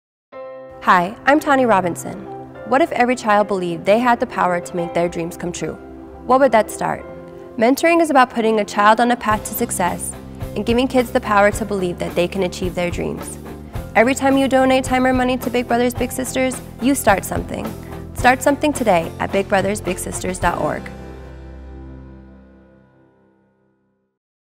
Tahnee Robinson for Big Brothers Big Sisters
Women's basketball player Tahnee Robinson, one of the first Native Americans ever selected in the WNBA draft, encourages you to Start Something today with Big Brothers Big Sisters. Every time that you donate your money or time, you're helping to Start Something big.